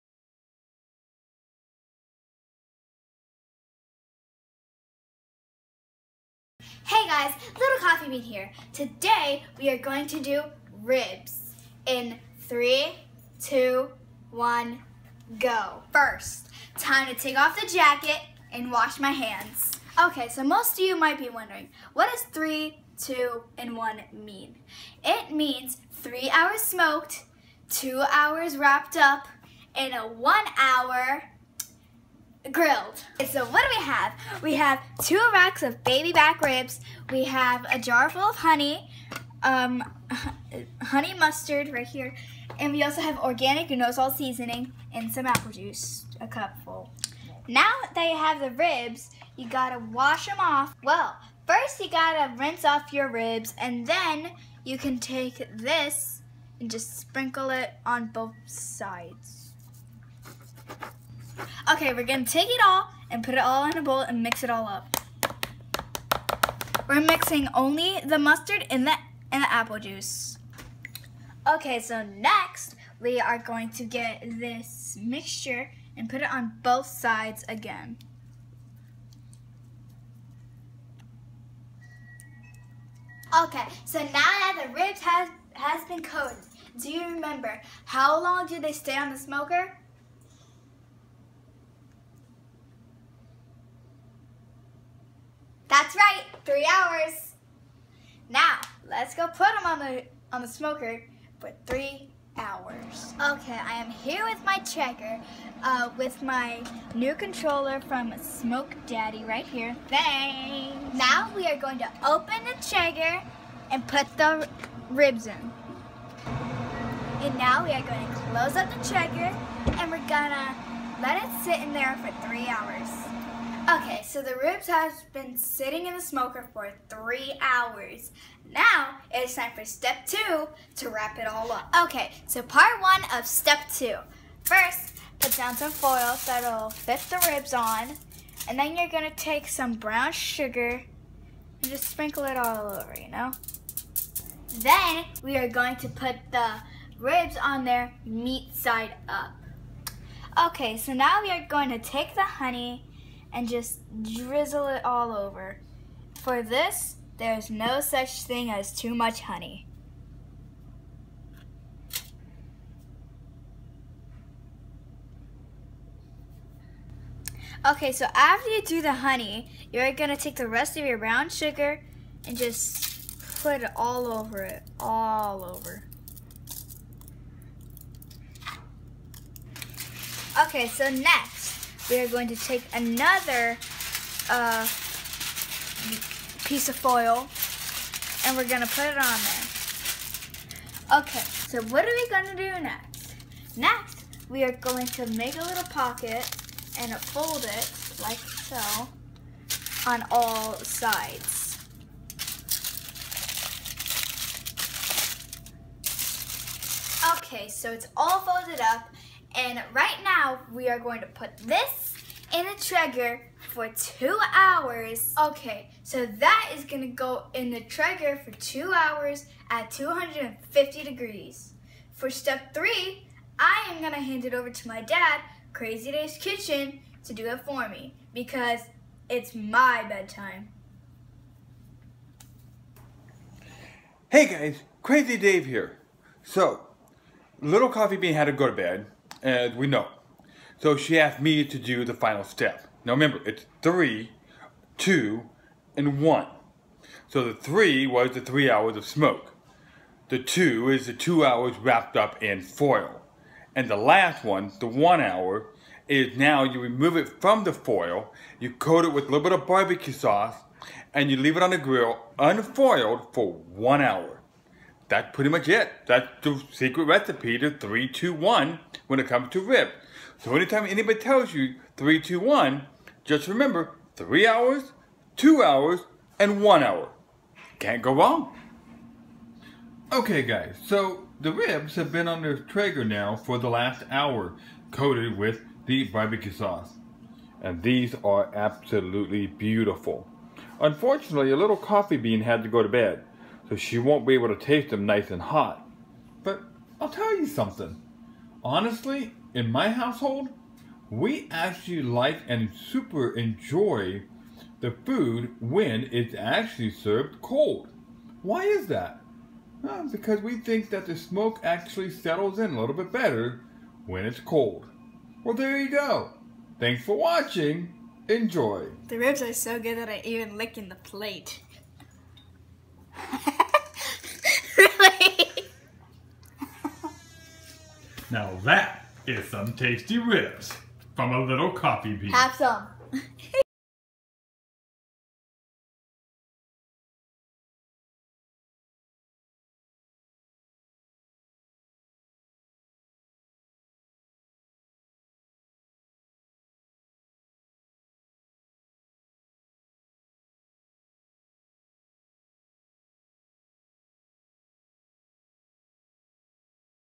321 Ribs so simple to make even I "A Little Coffee Bean " can make them.
A Little Coffee Bean shows how simple it is to make awesome lip smacking ribs with just 4 ingredients. She'll walk through step by step.